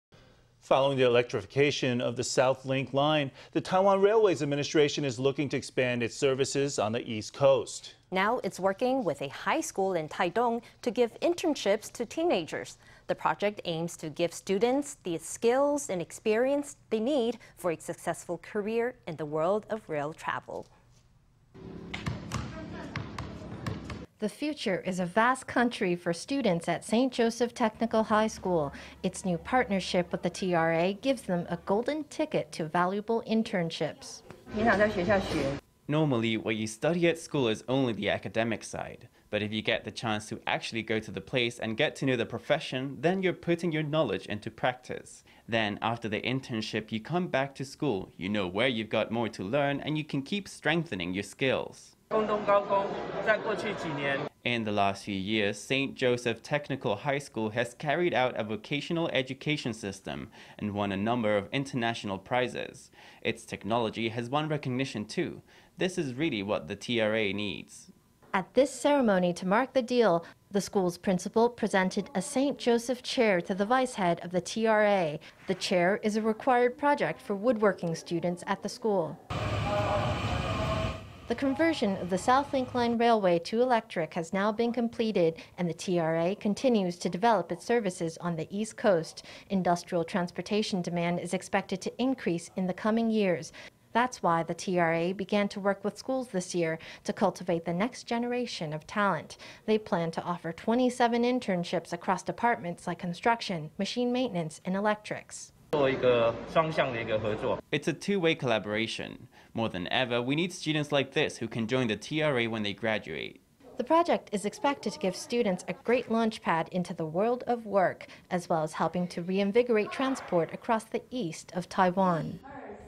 TRA gives internships to high-schoolers from St. Joseph Technical High School
Following the electrification of the South Link Line, the Taiwan Railways Administration is looking to expand its services on the East Coast. Now it’s working with a high school in Taitung, to give internships to teenagers. The project aims to give students the skills and experience they need for a successful career in the world of rail travel.

The future is a vast country for students at St. Joseph Technical High School. Its new partnership with the TRA gives them a golden ticket to valuable internships.

Huang Shu-ying
St. Joseph Technical High School principal
Normally what you study at school is only the academic side. But if you get the chance to actually go to the place, and get to know the profession – then you’re putting your knowledge into practice. Then after the internship, you come back to school, you know where you’ve got more to learn and you can keep strengthening your skills.

Feng Hui-sheng
Taiwan Railways Administration
In the last few years, St. Joseph Technical High School has carried out a vocational education system, and won a number of international prizes. Its technology has won recognition too. This is really what the Taiwan Railways Administration needs.

At this ceremony to mark the deal, the school’s principal presented a “St. Joseph chair” to the vice-head of the TRA. The chair is a required project for woodworking students at the school.

The conversion of the South Link Line railway to electric has now been completed, and the TRA continues to develop its services on the East Coast. Industrial transportation demand is expected to increase in the coming years. That’s why the TRA began to work with schools this year, to cultivate the next generation of talent. They plan to offer 27 internships across departments like construction, machine maintenance and electrics.

Feng Hui-sheng
Taiwan Railways Administration
It’s a two-way collaboration. More than ever, we need students like this who can join the TRA when they graduate.

The project is expected to give students a great launchpad into the world of work, as well as helping to reinvigorate transport across the East of Taiwan.